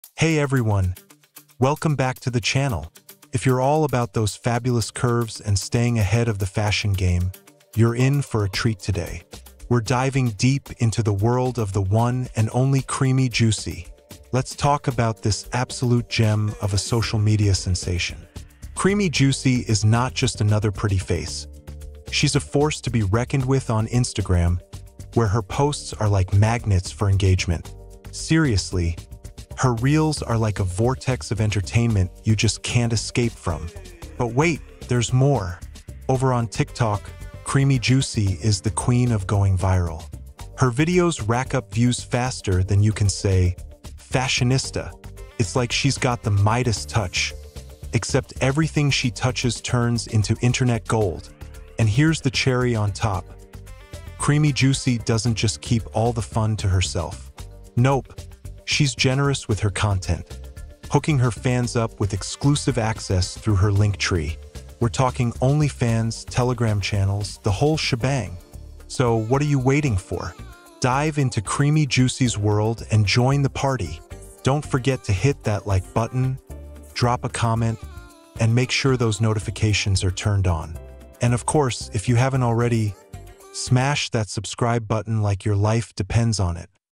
Creamyjuicy: Plus-Size Curvy Model Bio & Facts
Discover the fabulous Creamyjuicy, a stunning plus-size curvy model taking the fashion world by storm! 🌟 In this video, we explore her bio and share some fascinating facts about her life and career. Get ready to be inspired by her confidence and style. Don't miss out on this exciting journey with Creamyjuicy!